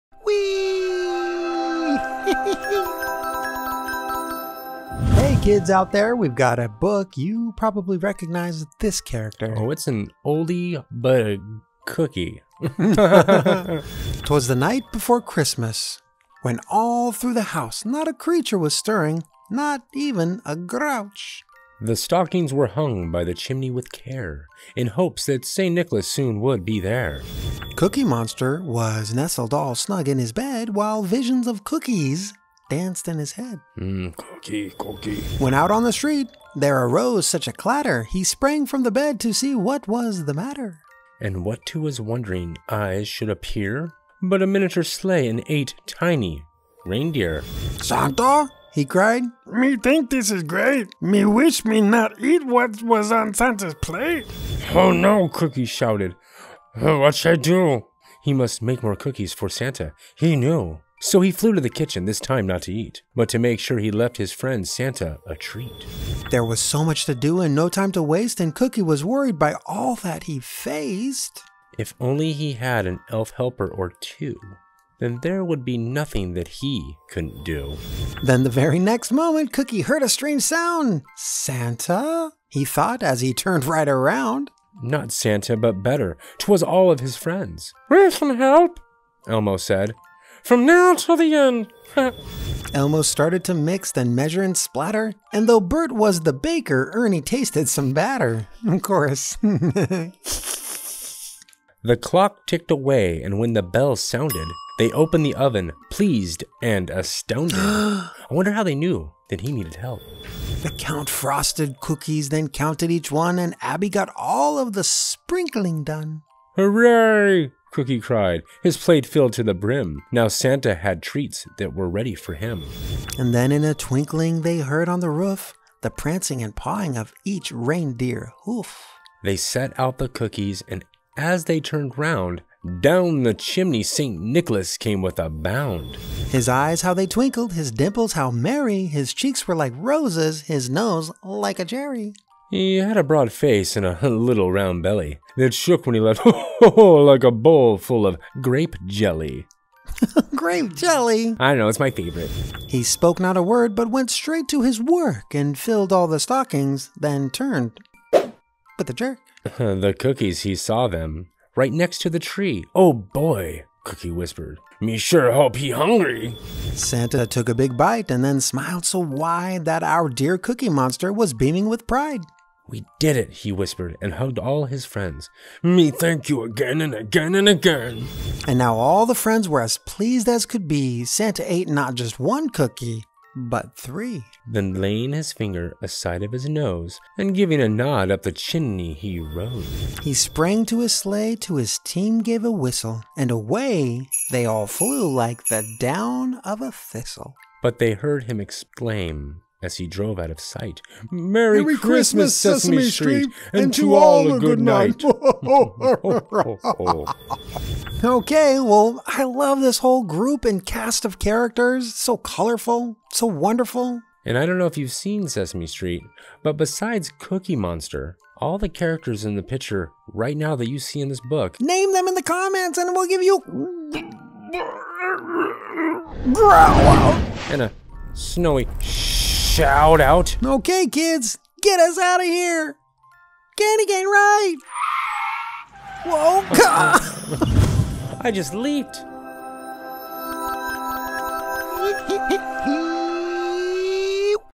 Twas the Night Before Christmas on Sesame Street 🎄 HOLIDAY BOOKS Read Aloud 🎄
We love you and thank you for your support!
Fuzzy and Snowy read the book called "Twas the Night Before Christmas on Sesame Street".
We hope everyone enjoys the story and the cursor for helping kids read and follow the story.
Thank you KidFunCo family.
Amazon link to some children's books